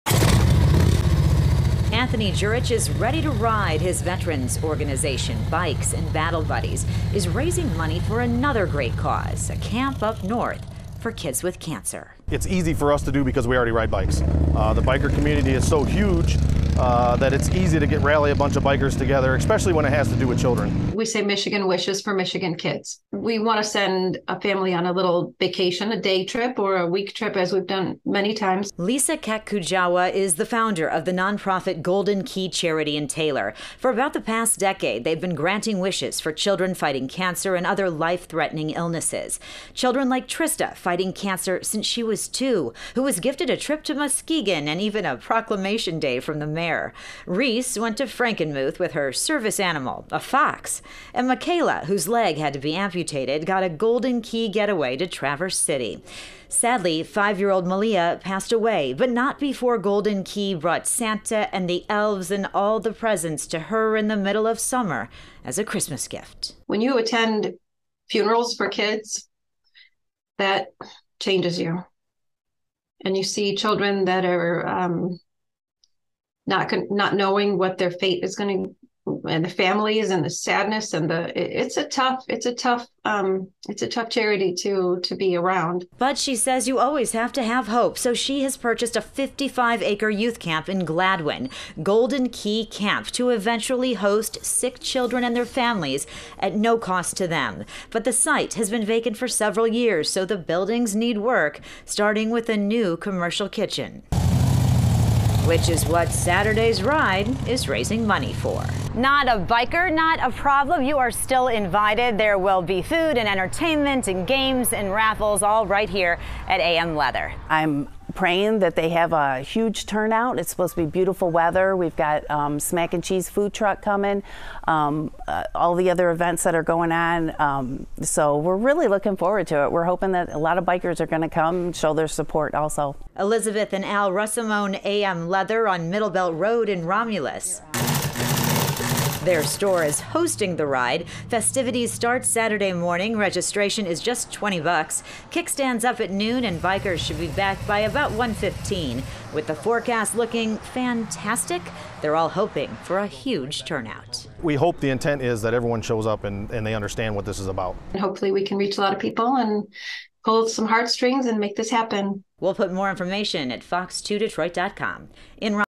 Nonprofit plans northern Michigan camp for kids with cancer
Nonprofit Golden Key purchased a camp in northern Michigan that will be converted into a camp for children with cancer and their families. On Saturday, motorcyclists are helping raise money to restore the buildings.